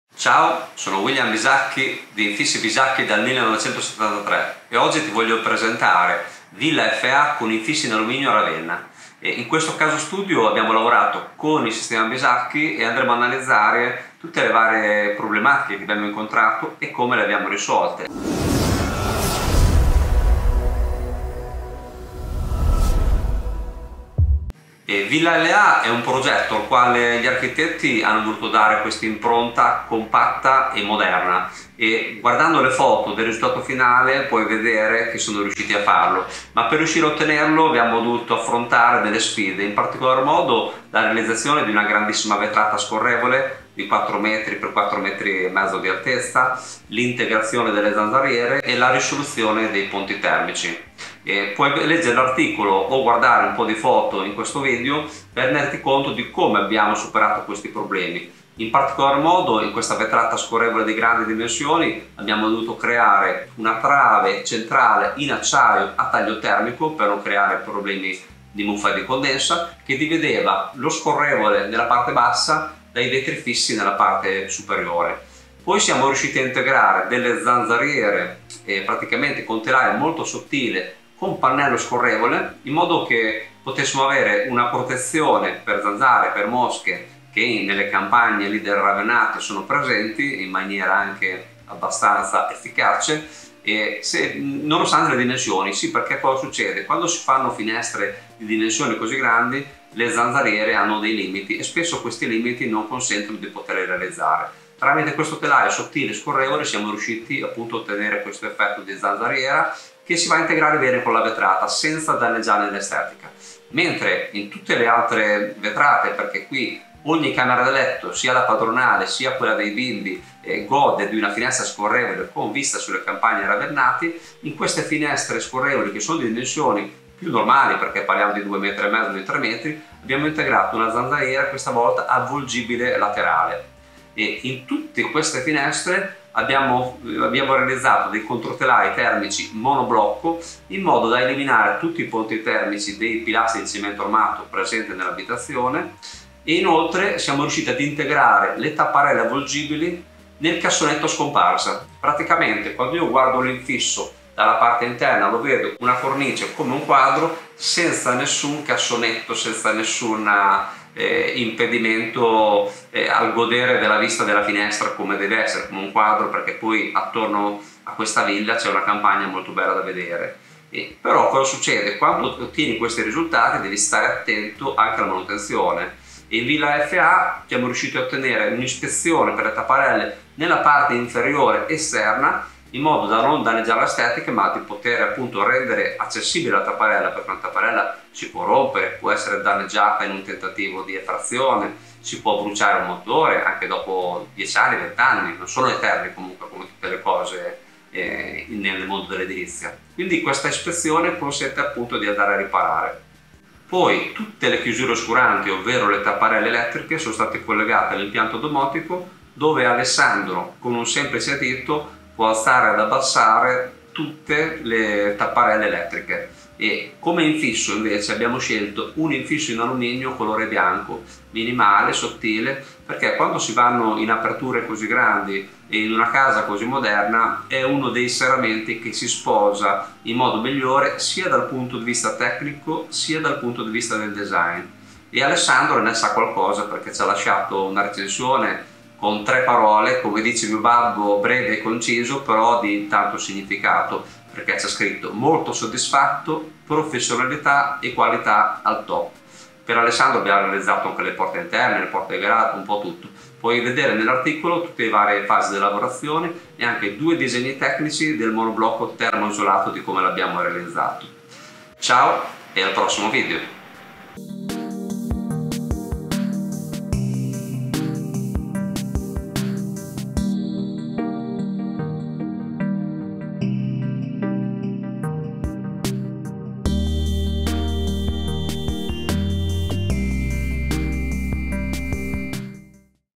Villa FA con infissi in alluminio a Ravenna
Villa FA è a Ravenna ed è stata realizzata con infissi in alluminio minimali di colore bianco puro e progettati con Sistemabisacchi®

Il progetto esecutivo è dello studio Archilab di Rimini , il quale è riuscito a dare compatto e minimalista come desiderato dal cliente ed il risultato è una villa che si fa notare nelle campagne di San Pietro in Vincoli (Ravenna).

▶️Gli infissi in alluminio

Per questo progetto Alessandro ha scelto infissi in alluminio a taglio termico bianchi perchè si sposavano perfettamente con lo stile moderno e minimale della sua villa, inoltre per alcune delle sfide che si presentavano erano la soluzione migliore dal punto di vista meccanico e di tenuta agli agenti atmosferici.

Le sfide da affrontare per questa villa a Ravenna sono state principalmente 3:

1- La realizzazione di una grandissima vetrata nella zona living
2- L’installazione di zanzariere integrate nelle grandi aperture
3- L’eliminazione di ponti termici
L’enorme vetrata della zona living sviluppa 4 metri di larghezza e bel 4,5 metri di altezza ed è stata realizzata con un unico scorrevole a 2 ante in alluminio minimale ed un sopraluce fisso diviso in 2 specchiature come lo scorrevole.

Per sostenere il peso delle enormi lastre di vetro di sicurezza abbiamo realizzato una speciale trave in acciaio a taglio termico per non avere nessun problema di muffa e condensa.

Come puoi notare nella prima foto, nonostante le incredibili dimensioni siamo riusciti ad integrare una zanzariera a pannello che scorrevole di fianco ai grandi vetri e rende molto pratica l’apertura, il telaio della zanzariera è molto sottile e si sposa con il design minimale della villa.

▶️ Vetrate scorrevoli per zona notte
È  il caso di dire “ad ognuno il suo scorrevole” perchè a Villa FA sia che i grandi che i piccoli hanno diritto ad una grande vetrata scorrevole in alluminio che permette ai suoi occupanti di rilassarsi guardando le campagne Ravennati circostanti.

Per queste vetrate abbiamo scelto vetrate scorrevoli alzanti che garantiscono uno scorrimento fluido e duraturo nel tempo.

Le vetrate sono completamente prive di guide a terra perchè abbiamo creato un sistema per affogarle nel pavimento ma mantenendo la massima tenuta all’acqua.

Anche qui, le zanzariere sono integrate ma con una soluzione diversa, abbiamo utilizzato delle avvolgibili laterali a scomparsa e senza guida a pavimento.

▶️ Le chiusure oscuranti

Per oscurare gli infissi in alluminio di villa FA si è optato per delle pratiche tapparelle elettriche ma a scomparsa totale, infatti il cassonetto è stato integrato nella parte con un sistema a monoblocco che ne ha massimizzato l’isolamento termico, acustico e azzerato i ponti termici.

Le tapparelle elettriche sono comandate dalla domotica e Alessandro quando esce di casa può chiudere tutte le tapparelle con un solo tasto.

Abbiamo garantito la manutenzione di tapparelle e motori con un ispezione esterna in modo da avere le pareti interne completamente lisce e pulite come voleva Alessandro e senza il rischio di perdere prestazione acustica con dei giunti interni.

▶️ Controtelai termici per infissi in alluminio

Oltre ai monoblocchi abbiamo realizzato i controtelai termici per i tagli di luce a vasistas senza tapparelle, questi controtelai sono stati realizzati in legno osb ed epe con profili reggi-rasatura.

Questi accorgimenti ci hanno consentito di risolvere i ponti termici dei pilastri in cemento armato e rendere più agevole il lavoro dei cappottisti.